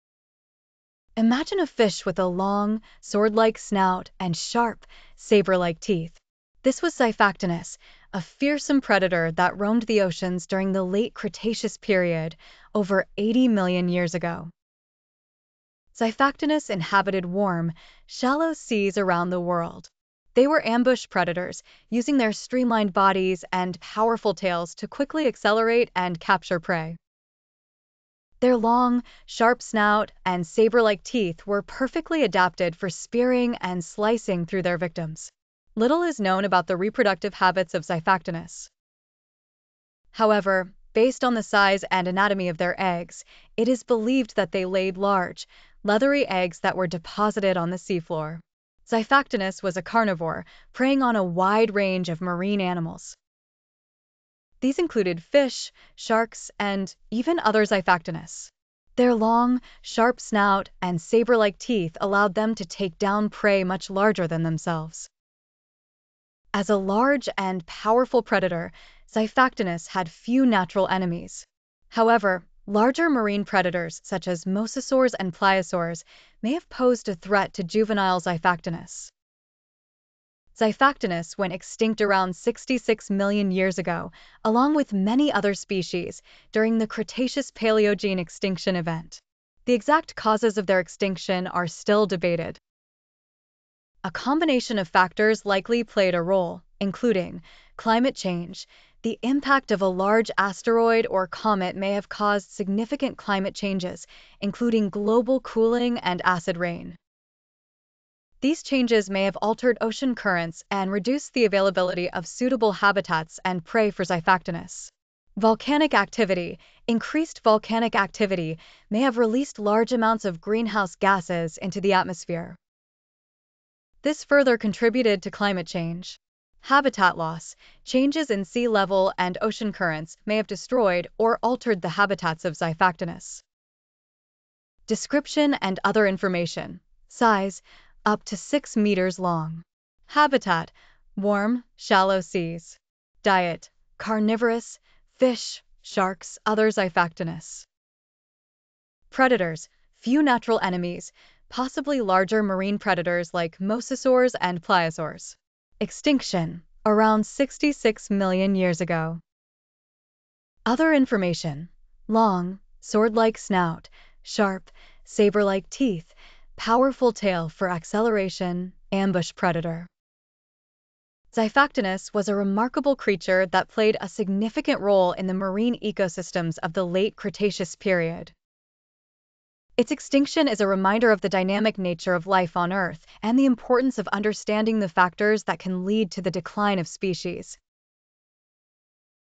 Xiphactinus: The Fearsome Predator of the Late Cretaceous
Xiphactinus, a formidable predator from over 80 million years ago, inhabited warm seas worldwide. With a sword-like snout and saber-like teeth, it hunted fish, sharks, and even its own kind. Extinct for 66 million years, its demise may be linked to climate change, volcanic activity, and habitat loss.